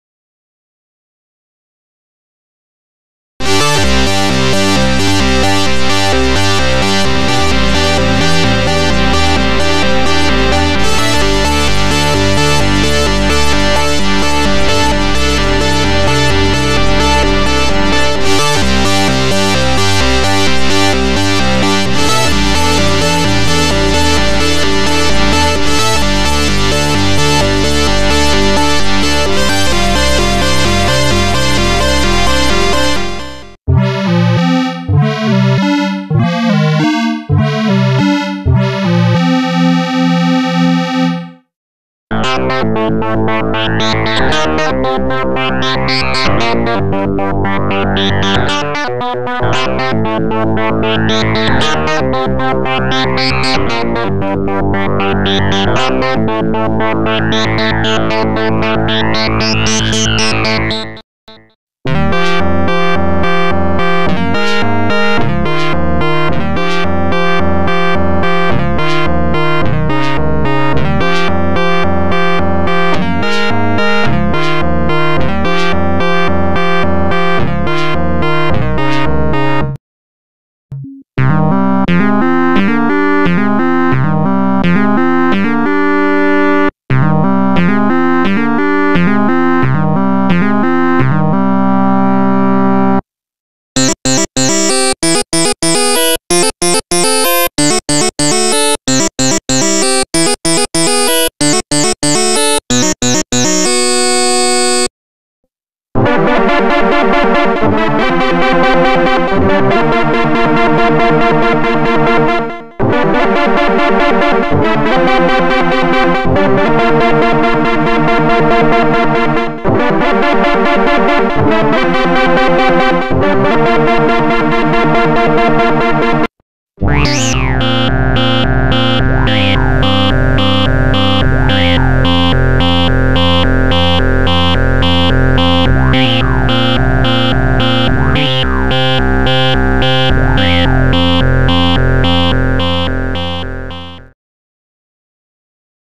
Cisco Jugo's Casio CZ Pack #1 Demo (no effects)
A collection of 16 Patches for Casio CZ series synths or VirtualCZ!

These sounds use envelopes, square wave vibrato or both to play short arpeggios, pitch shifting sequences and even 2 oscillator chords.
The pack includes 16 Individual patch files in .syx format and a VST .fxb file that loads the 16 patches into VirtualCZ.
Programmed in a CZ-101 and compatible with all CZ series synths
Includes a DCO-Vibrato to Pitch table to make it easier to tweak the sound or come up with variations.